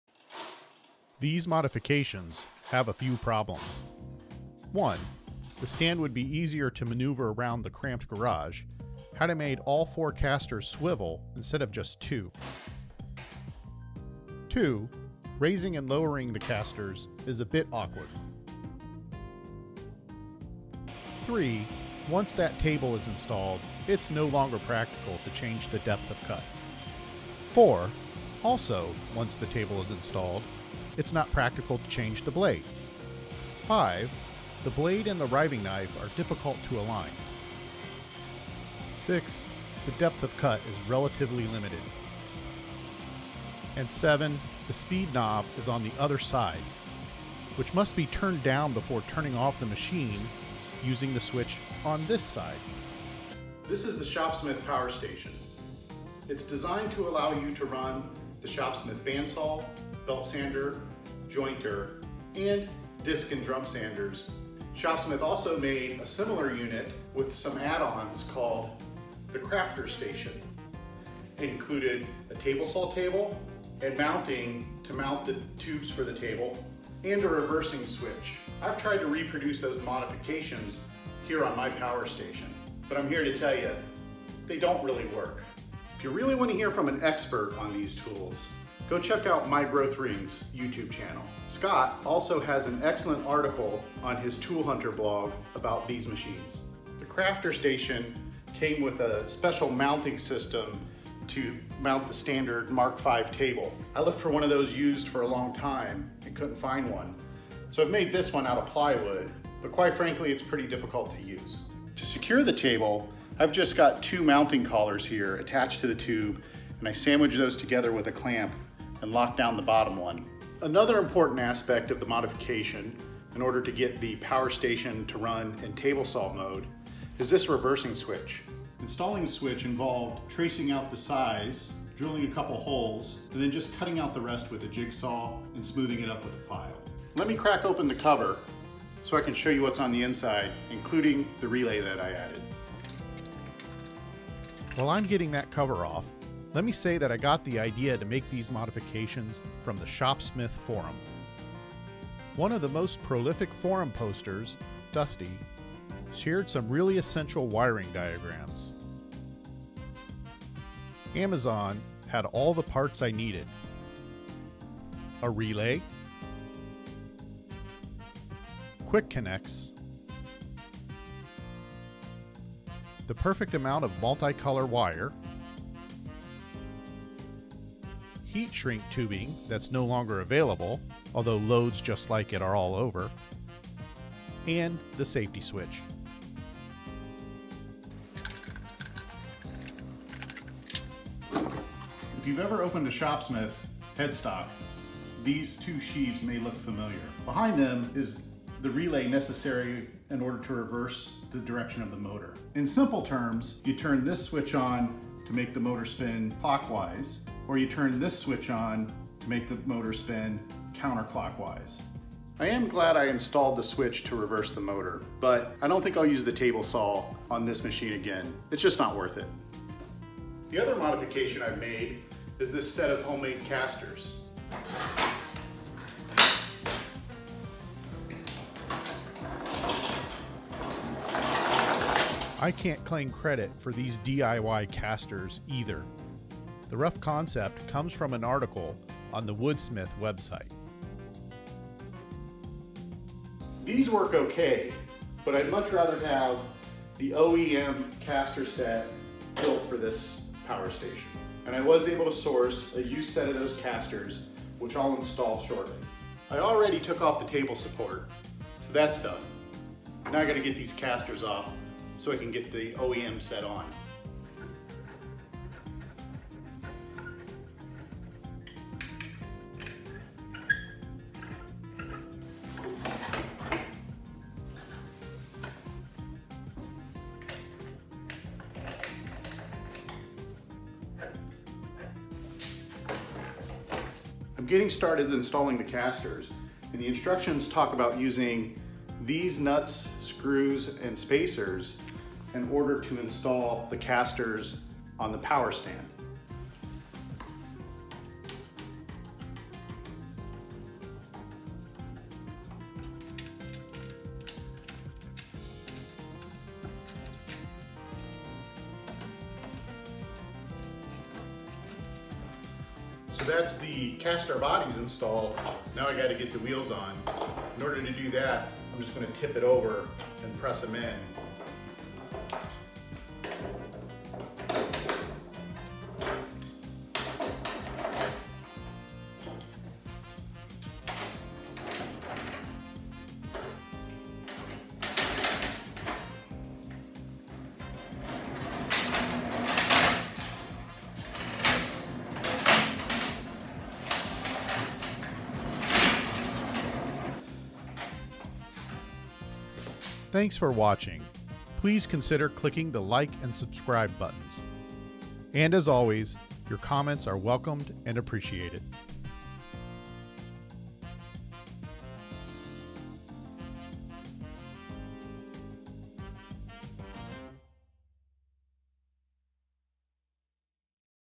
Woodworking Tool Stand Hack Fail: I Regret These Modifications
Shopsmith Power Station to Crafter's Station conversion is a bust. DIY tools stand casters are too difficult to use. I correct these mistakes to restore the tool stand to proper working order.

Links:
Tool-Hunter Carfter's Station blog post